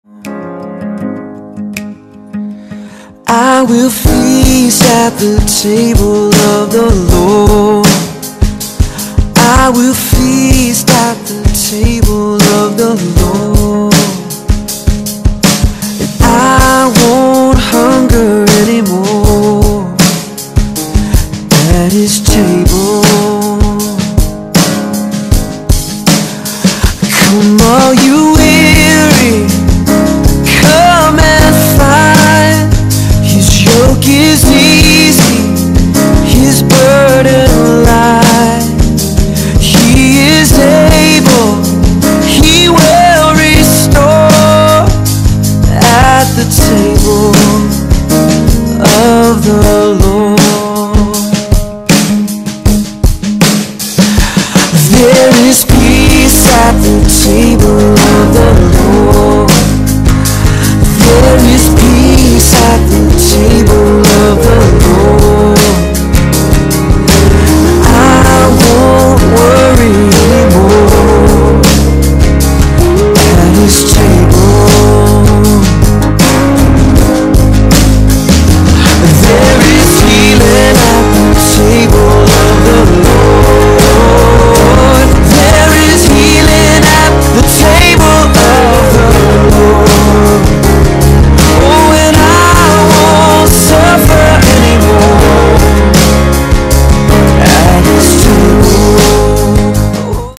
Passover Seder (Triclinium format) with Jerusalem University College
Easter-time special event for the Jerusalem University College spring semester, hosted by the Cultural Backgrounds class each year. This is the 2015 event.